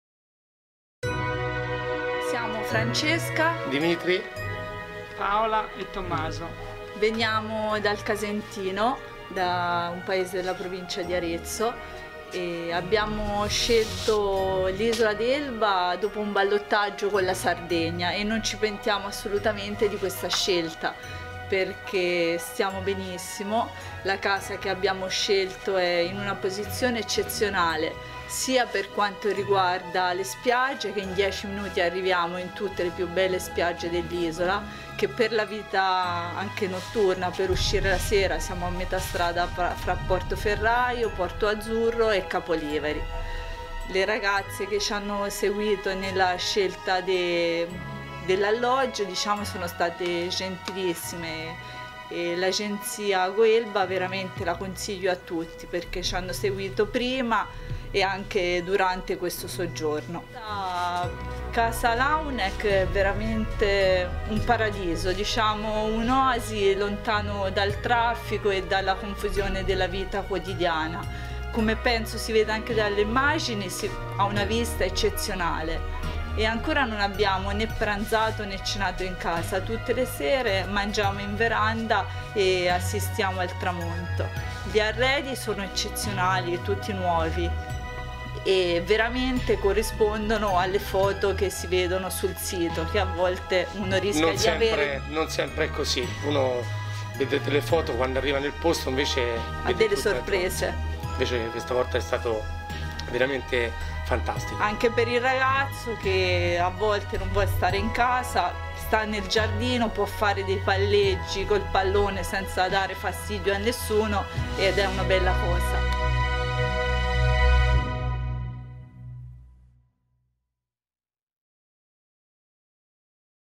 Villa in un luogo fantastico all'isola d'Elba
Una villa nuova in un punto panoramico con una strepitosa vista mare all'isola d'Elba.
Nel video la testimonianza di una famiglia che vi ha soggiornato in luglio: Francesca, Dimitri, Paola e Tommaso, entusiasti della bellezza del luogo e pienamente soddisfatti del servizio di goelba.it... un piacere per noi che lavoriamo sodo alla soddisfazione degli ospiti.
Se vuoi vedere la scheda con foto, prezzi, servizi e un bel video con drone di Villa Launeck all'isola d'Elba, clicca questo

Per altri Video come questo